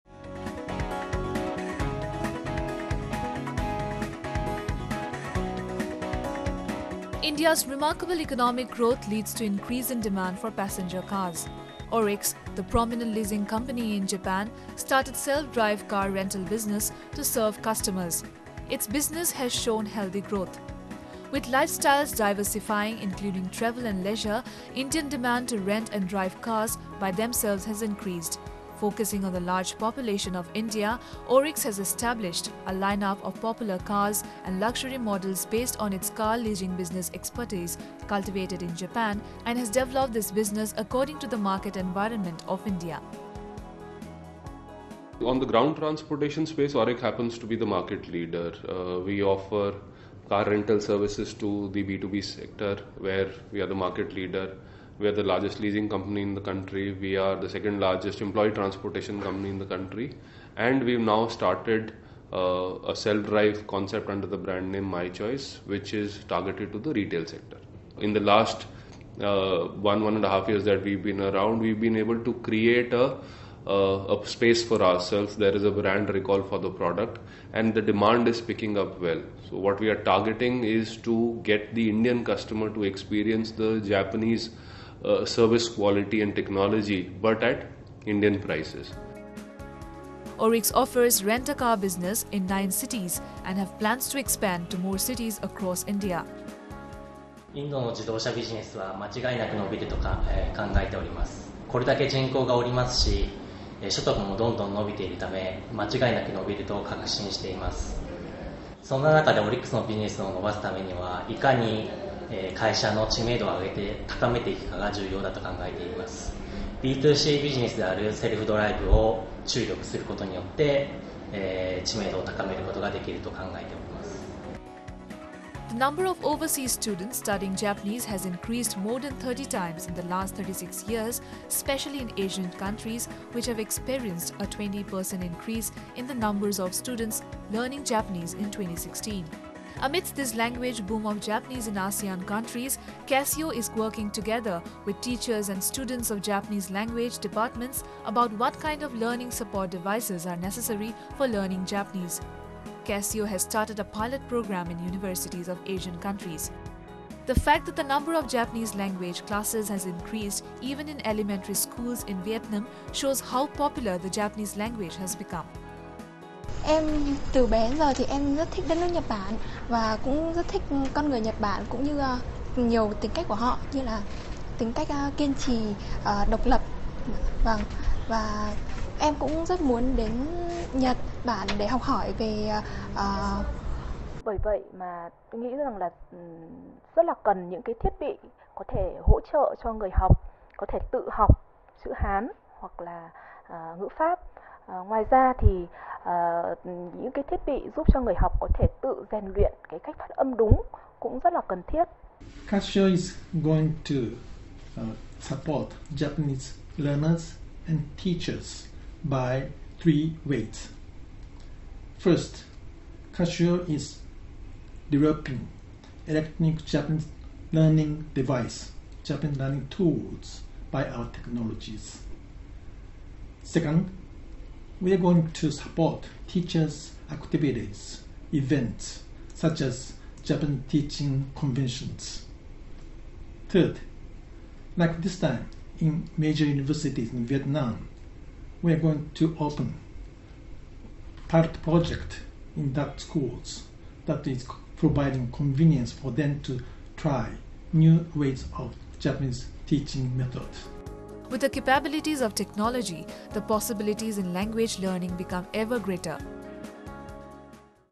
Japanese firm starts self-drive car business in India - ANI News
New Delhi/Vietnam, Sept 07 (ANI): India’s remarkable economic growth leads to increasing demand for passenger cars. Orix, the prominent leasing company in Japan, started Self-drive Car Rental business to serve customers.